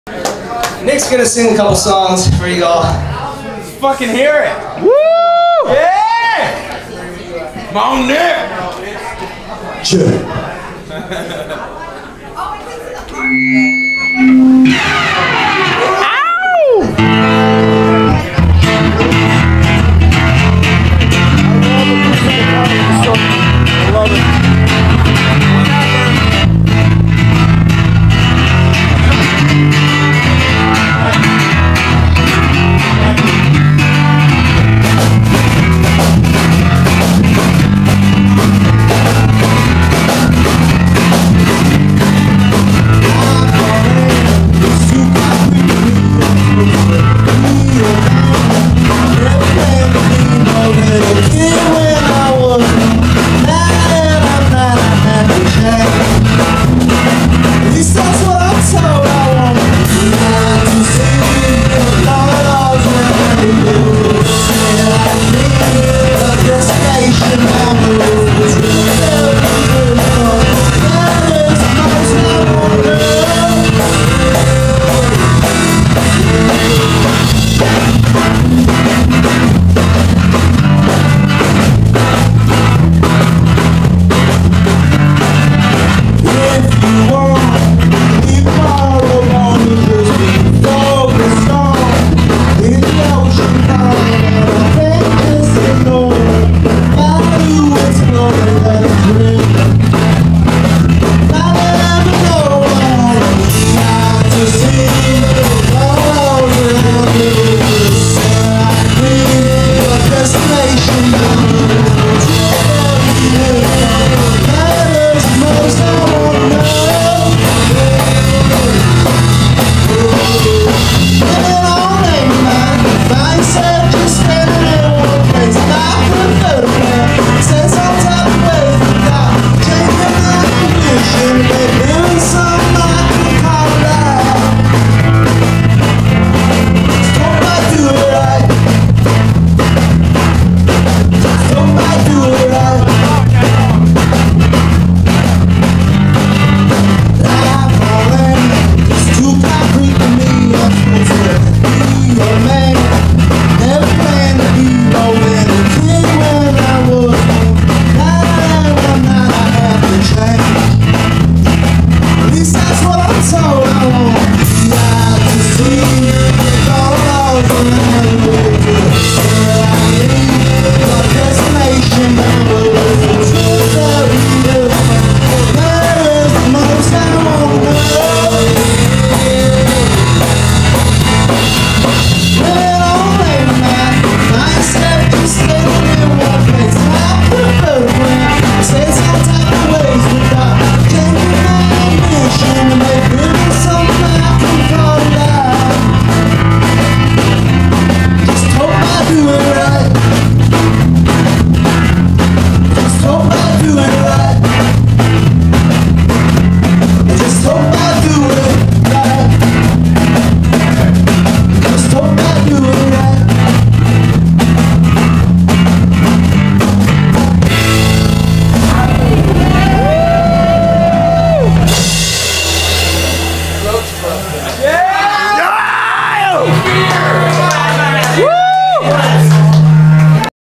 Dolly llama Partons Live - Unknown song 7
Live at the brass rail Very dark environment so the video is rather dark... but hey it is good music.